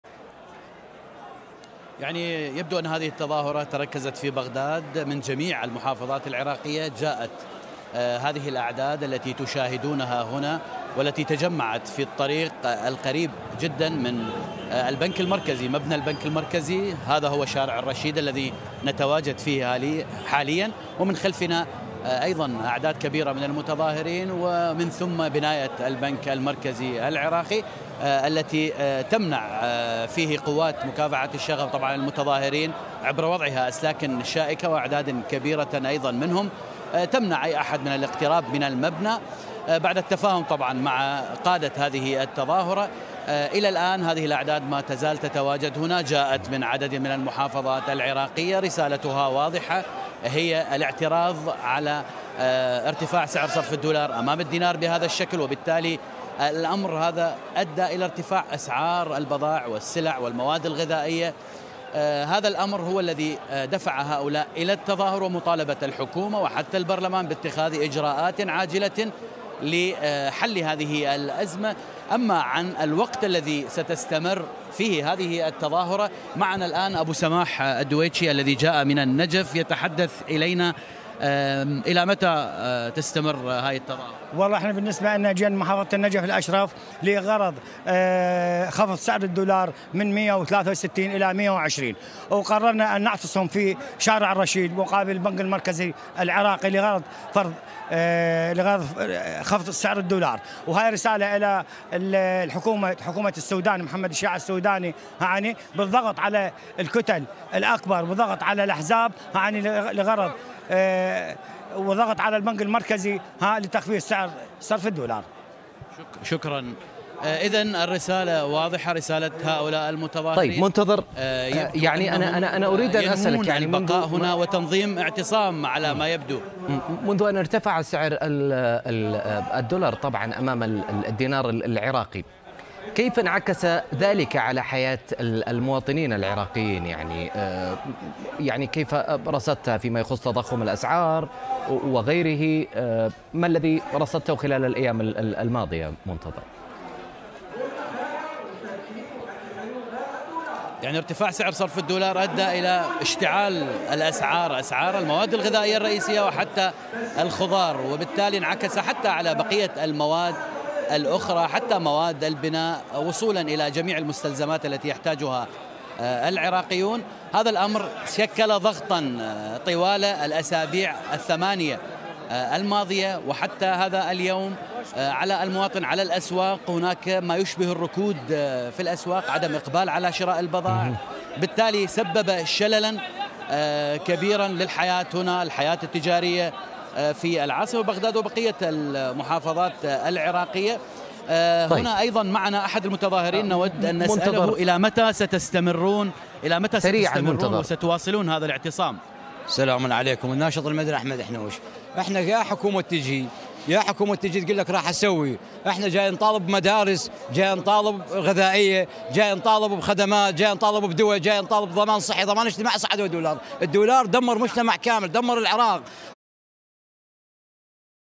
استمرار التظاهرات أمام البنك المركزي احتجاجا على تراجع الدينار
العراق.. استمرار التظاهرات أمام البنك المركزي احتجاجا على تراجع الدينار.. ومراسل العربية: ربما تتحول التظاهرات إلى اعتصام